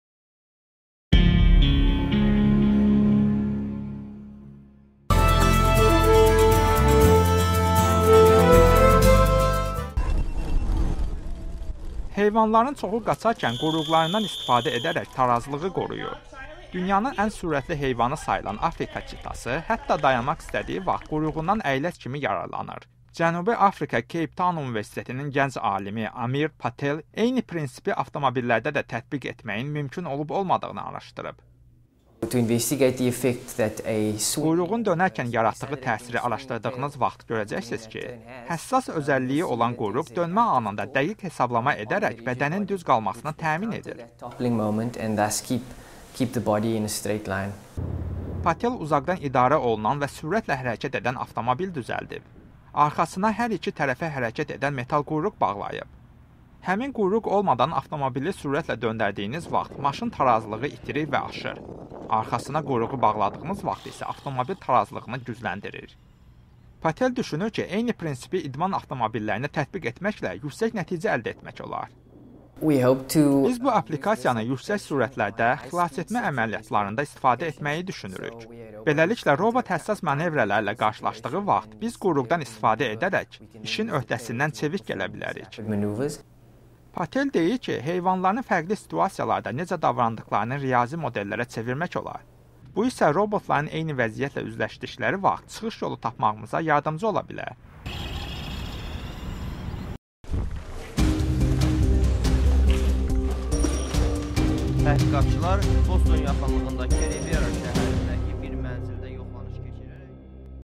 Alimlər quyruqla tarazlıq arasında əlaqə aşkar edib
Əgər sən döngədə velisopedinin sürətini artırsan, tarazlığını qorumaq üçün yana əyilməlisən. Bir çox sürətlə hərəkət edən heyvanlar eyni şeyi edirlər, və çox vaxt quyruqlarından əlavə kömək alırlar. Cənubi Afrikalı gənc alim robotların sürətlə hərəkət edə bilməsi üçün bu məhfumu kəşf edib.